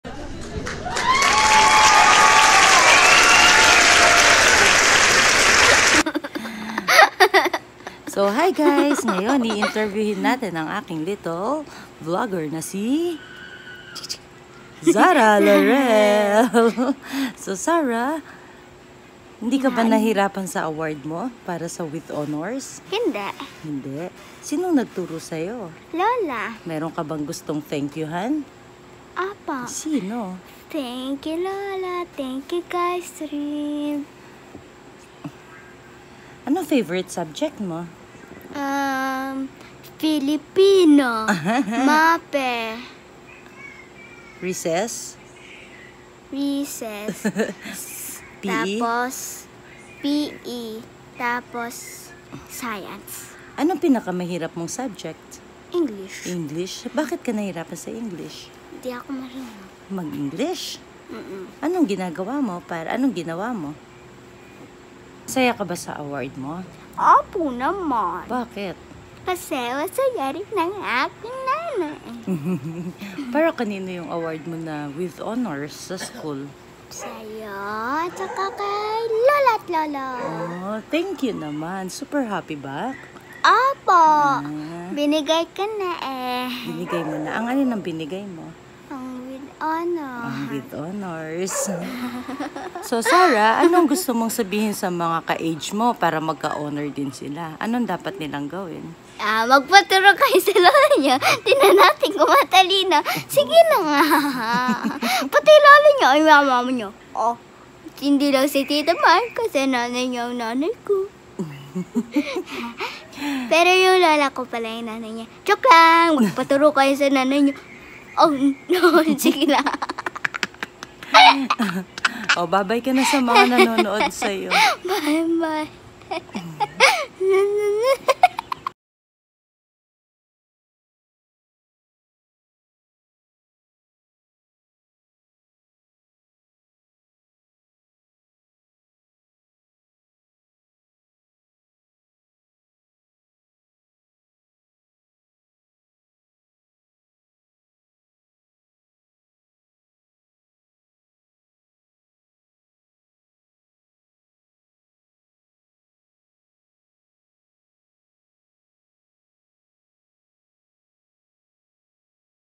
short interview with Zahra (with honors)
congratulations  Zahra and thank you for the wonderful gift you have given mama, lola and lolo.  May you always strive hard in everything you do while enjoying yourself doing the things you love to do.  God bless you always anak.